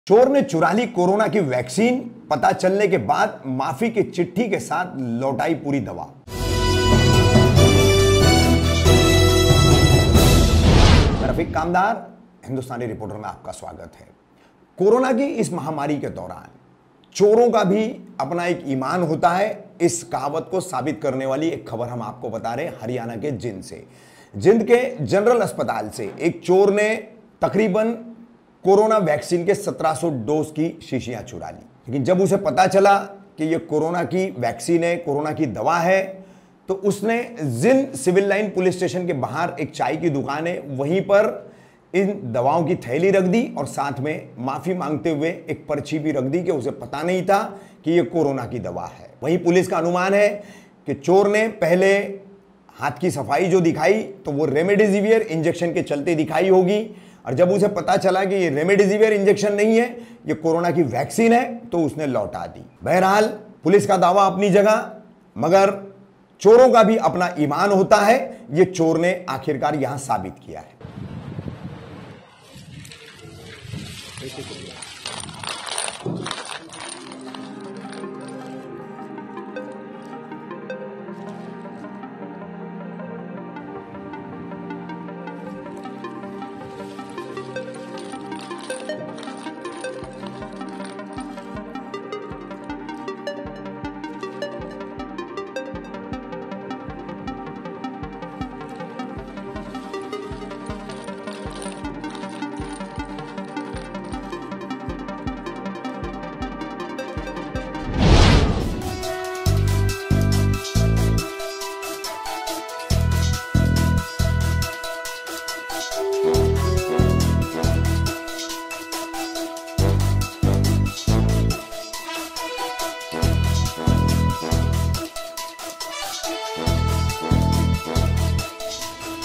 चोर का ईमान जाग गया चुराई हुई कोरोना वैक्सीन माफी की चिट्ठी के साथ वापस लौटाई | Hindustani Reporter
चोर का ईमान जाग गया

चुराई हुई कोरोना वैक्सीन माफी की चिट्ठी के साथ वापस लौटाई

Mic We Use